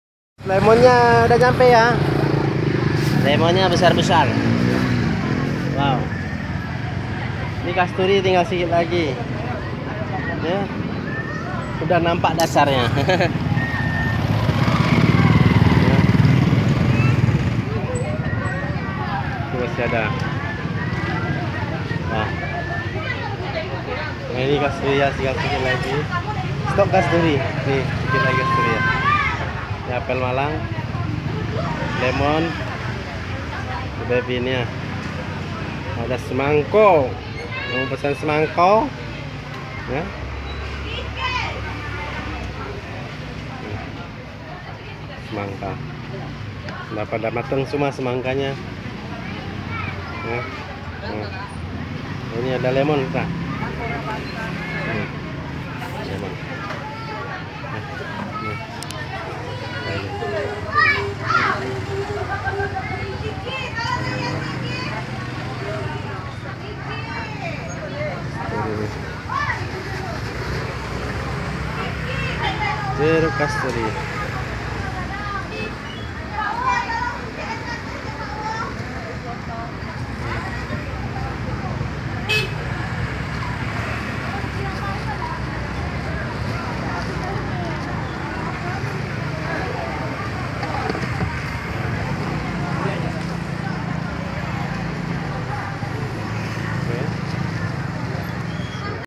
Bg zakaria aceh menstockan jeruk kasturi dan jeruk lemon untuk bulan ramadhan karena buat berbuka
Zakaria Aceh Group adalah distributor utama buah buahan segar dan langka kota duri riau hp/wa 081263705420